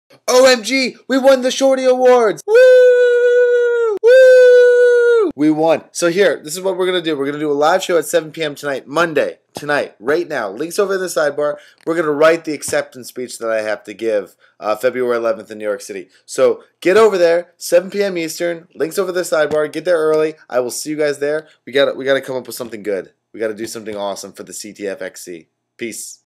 WE WON! YAY!
looks like we wont the shorty awards for comedy! woooooo! help us write the acceptance speech tonight at 7pm eastern on blogtv